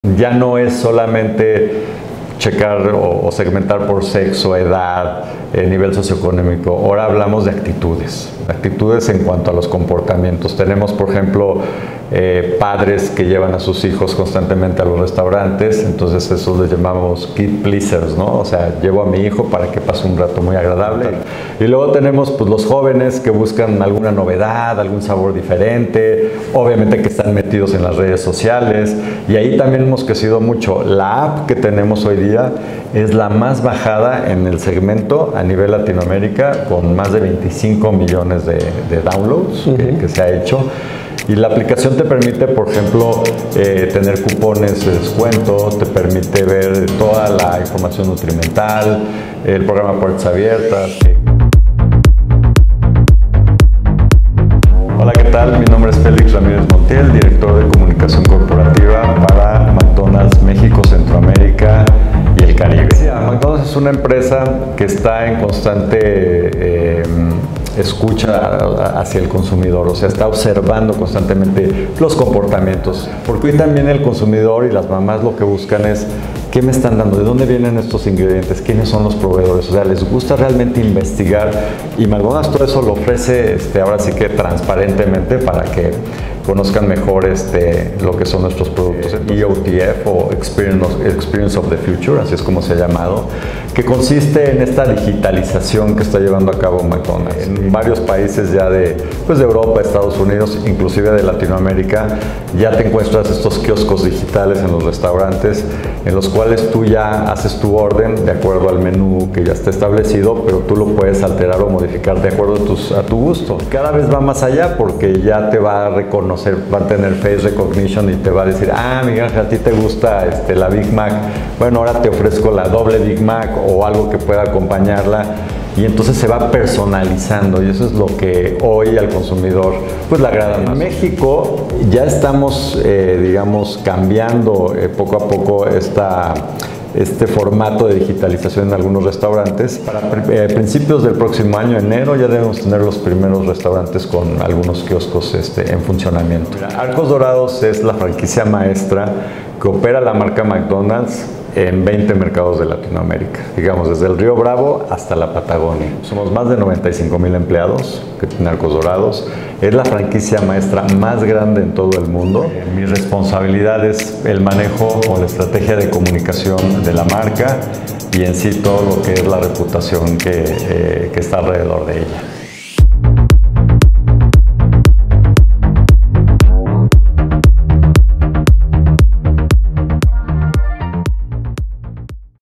McDonald’s / Secretos para digitalizar tu negocio / Emprendedores
Ya no se trata de segmentar por edad o sexo ahora se habla de actitudes.
Habla Félix Ramírez, director de comunicación corporativa de McDonald’s Arcos Dorados) para México, Centroamérica y el Caribe, con Miguel Pallares, periodista de negocios.

Periodismo de Negocios · Noticias de Negocios